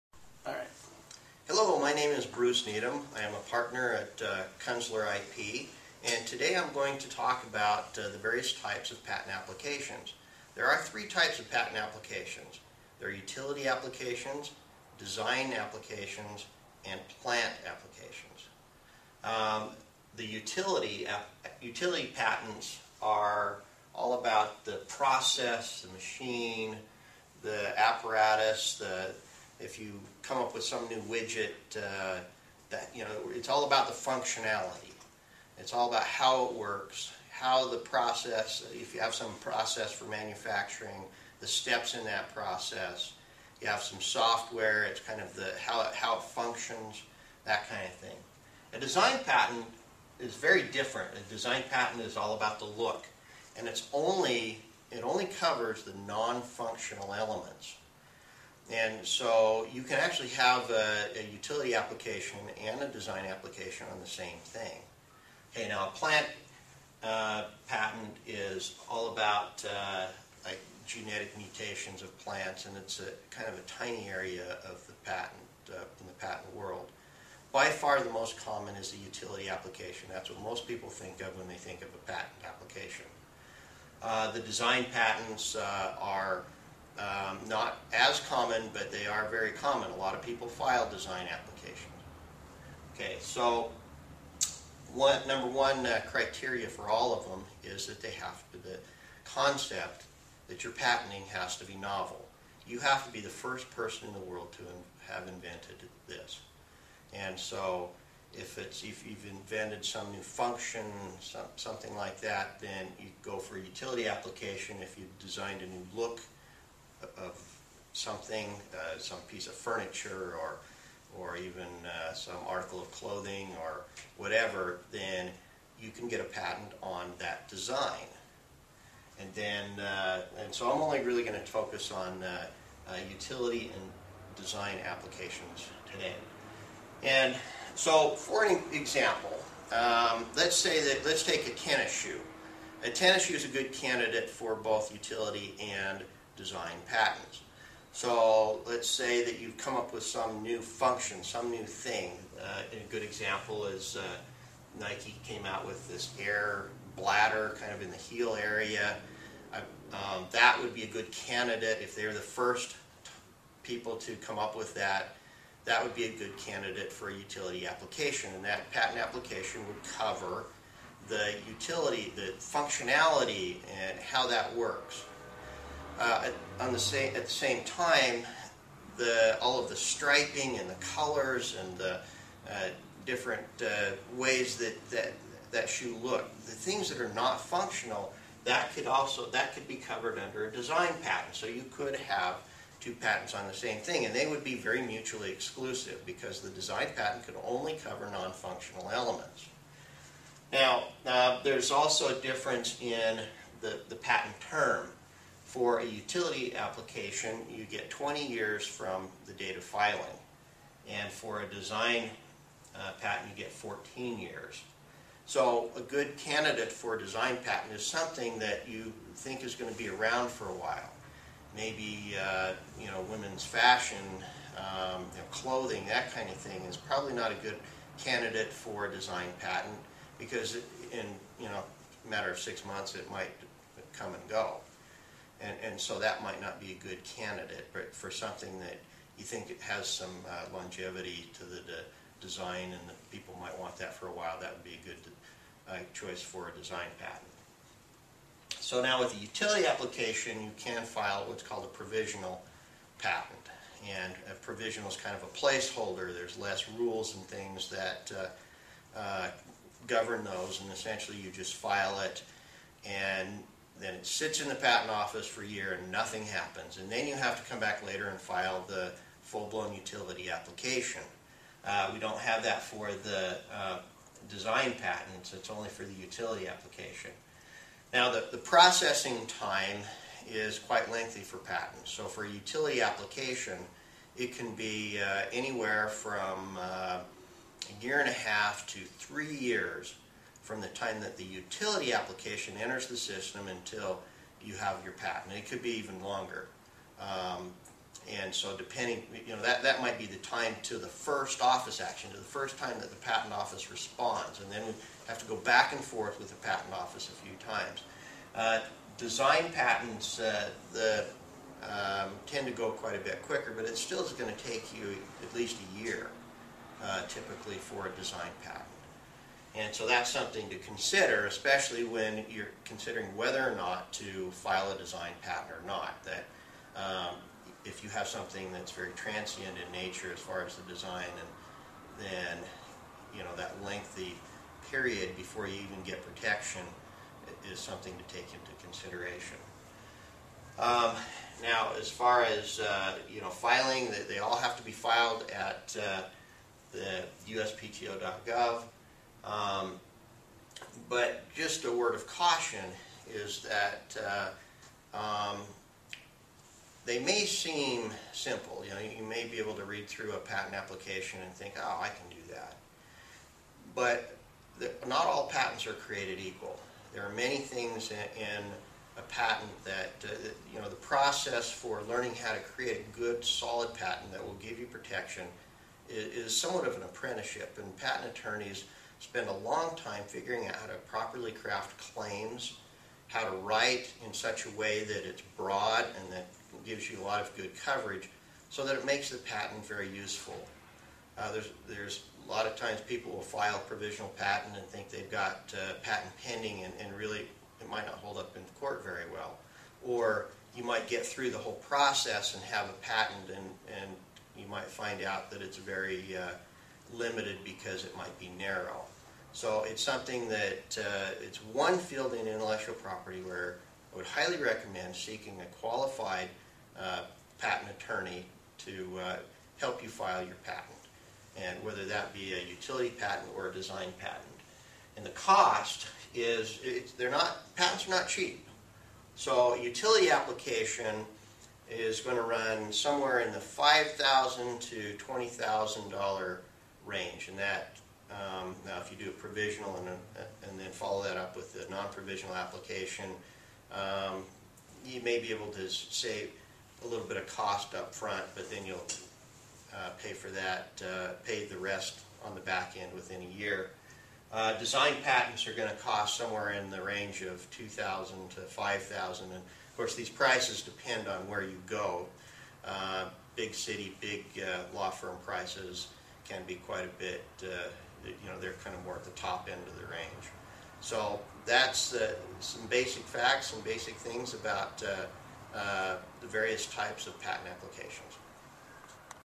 Utility vs Design Patents
What are the various types of patent applications? What are the differences? How do I decide which type of patent to use?